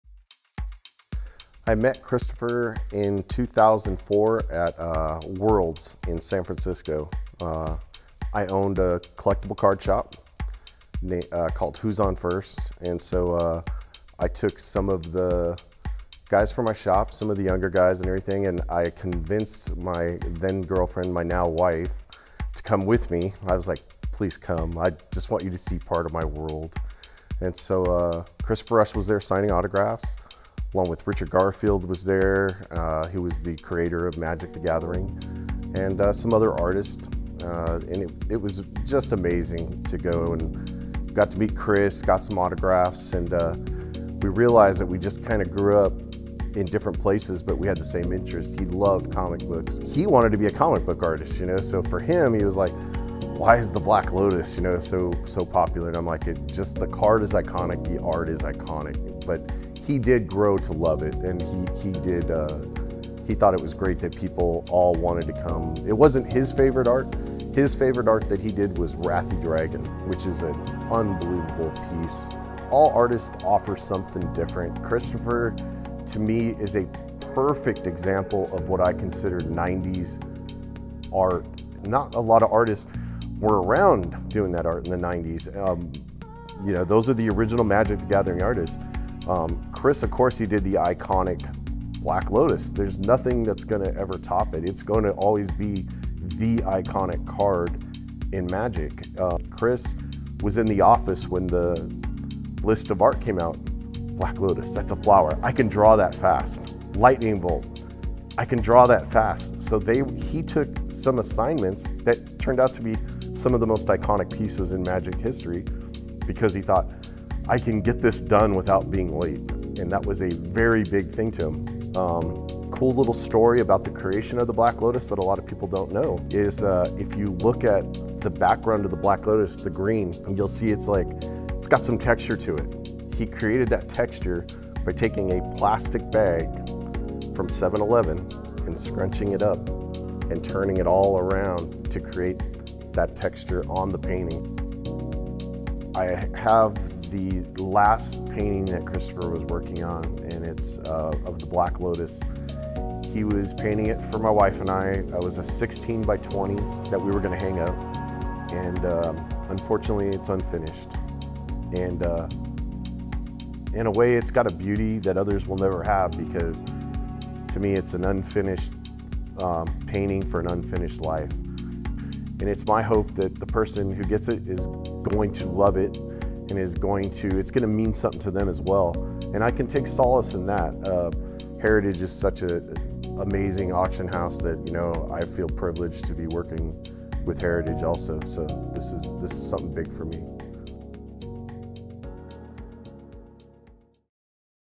‘Magic: The Gathering’ Original Art, Unfinished Black Lotus Painting In Play At Heritage Auctions
For many collectors, who you know can be every bit as important as what you know.
Consider the case of Jeff Ferreira.

The owner of Who’s On First, a collectible cards shop in Dublin, California, took his girlfriend and some of his staff to the Magic: The Gathering World Championship in San Francisco in 2004; his staffers already were immersed in the trading card games community, and he wanted his now-wife to see another part of his life.

While there, the group met Magic: The Gathering creator Richard Garfield and an artist named Christopher Rush.

Watch an exclusive interview with Ferreira on his collection.